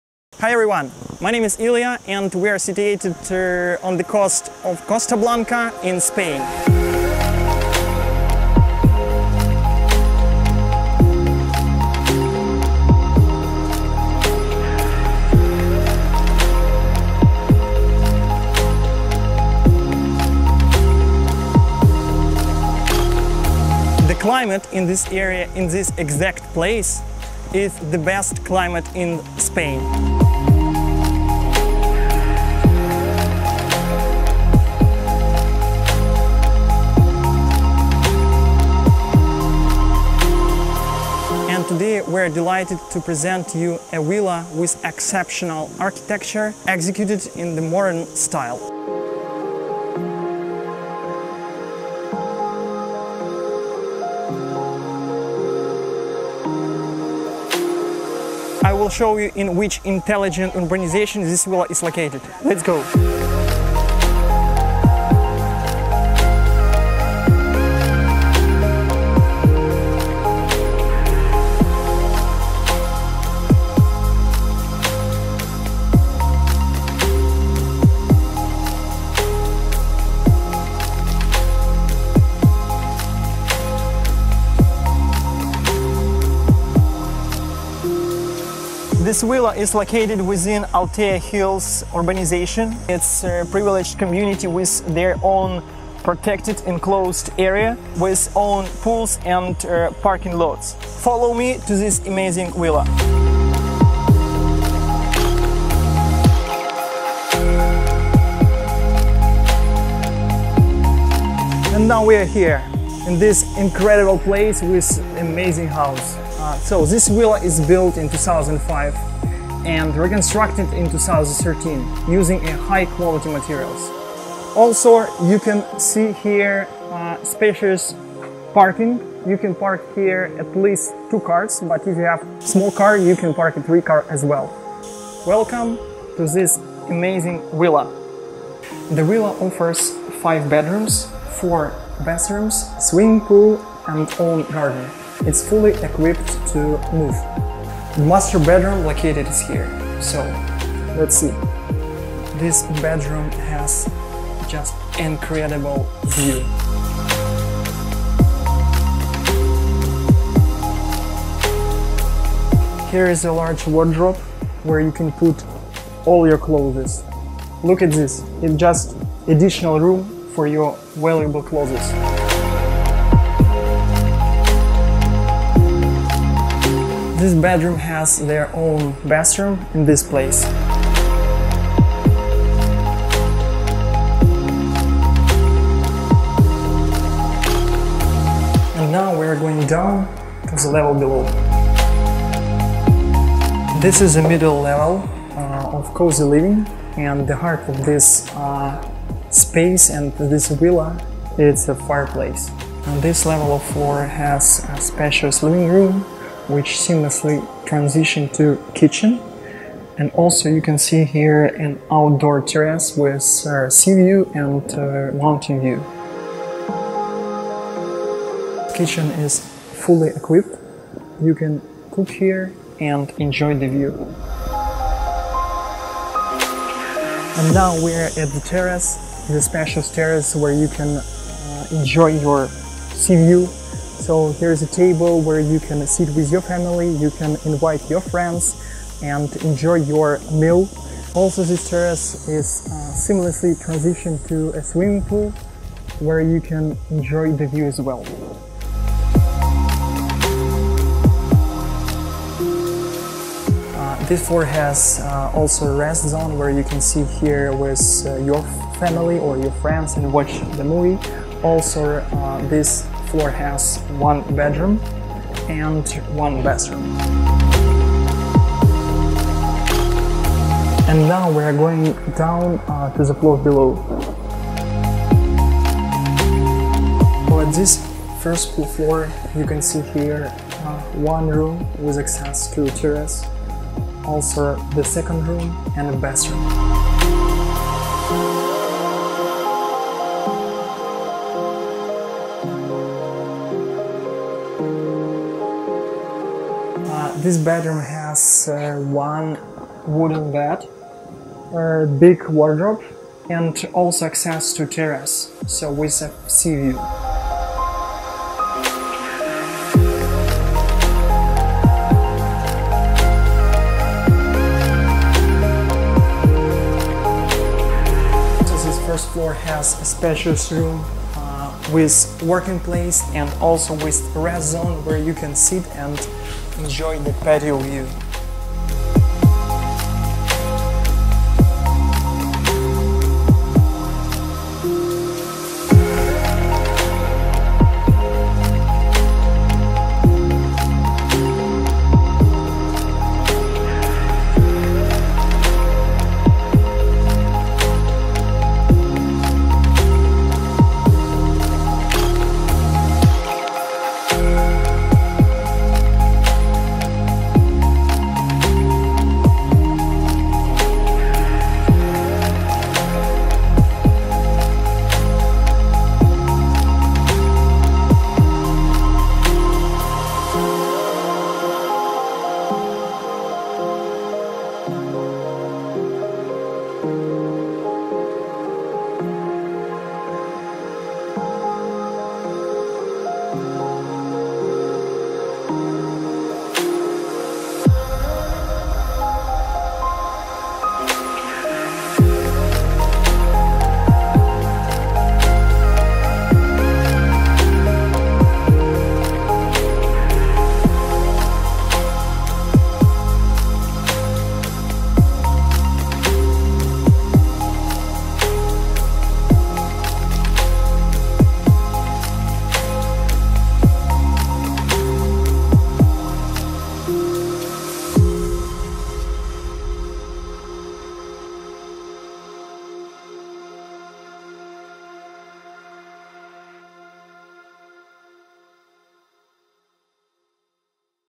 Villa House in Spain Altea Hills Alicante Costa Blanca with excellent views of the Mediterranean Sea
Exclusive Villa with three residential floors in the closed complex Altea Hills.

With contemporary architecture, the highest quality materials and exclusive furniture. The property has been renovated in 2013.

One of the advantages of living in this villa is that it is situated with the most beautiful panoramic views of the sea and the mountains.

The villa has a total constructed area of 450 square meters, has 5 large bedrooms and 4 bathrooms, swimming pool, private garden.

This is a new level of a cozy house with the use of a high-tech air conditioning system and a heating system independent of the fireplace.

On the upper floor there is a spacious hall and a bedroom with a private bathroom and a large unique dressing room with sea views.

The most outstanding space on the second floor of this elegant Villa is the spacious living room with kitchenette that connects to the outdoor terrace. The panoramic terrace with swimming pool offers spectacular views of the sea and the mountains.
A good idea to enjoy dinners with friends or family and took a breath of the fresh sea air.

On the ground floor there are 2 Bedrooms with suite bathroom, terrace with beautiful sea views. There is also a large work area and adaptable to your configuration needs, with access to your own patio.

We invite you to enjoy life in this luxury Villa where every moment becomes a value.